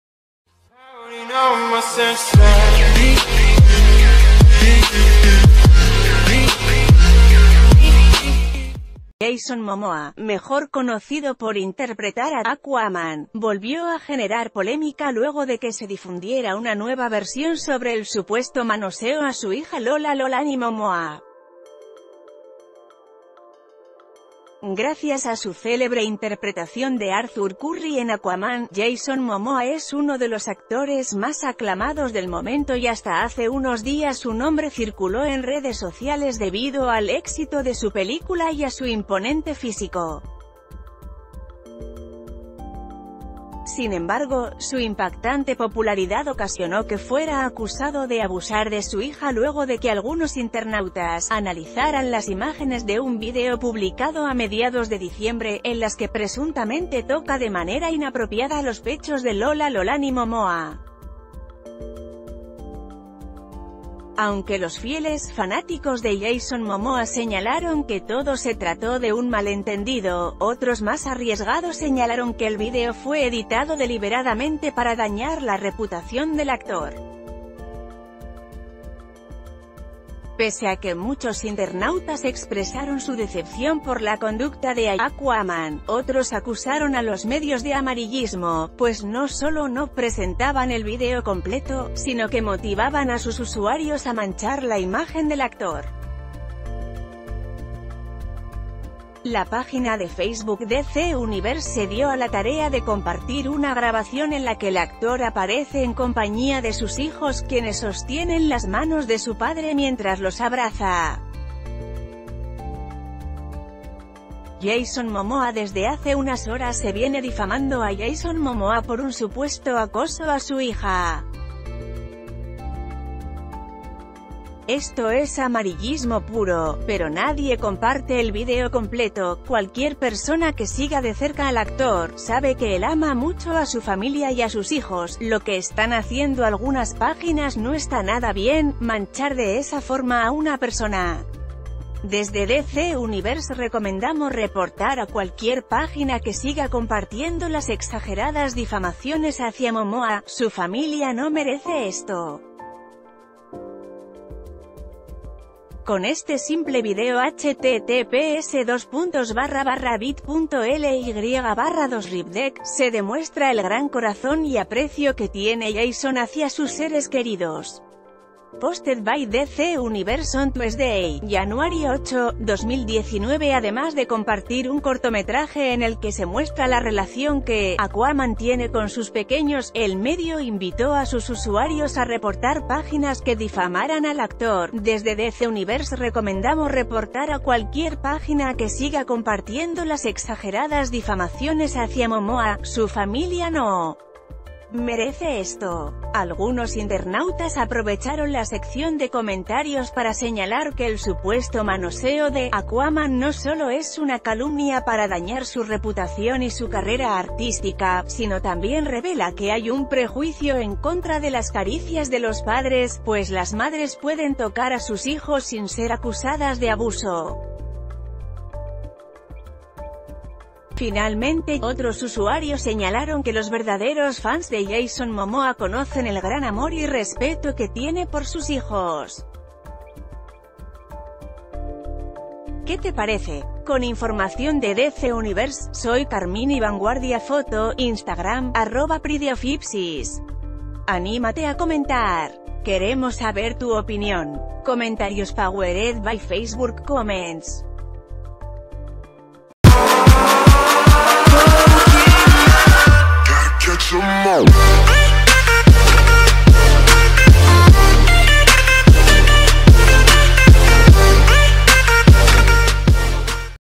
Difunden nueva versión del supuesto manoseo de ‘Aquaman’ a su hija
Jason Momoa, mejor conocido por interpretar a ‘Aquaman‘, volvió a generar polémica luego de que se difundiera una nueva versión sobre el supuesto manoseo a su hija Lola Lolani Momoa. Gracias a su célebre interpretación de Arthur Curry en Aquaman, Jason Momoa es uno de los actores más aclamados del m...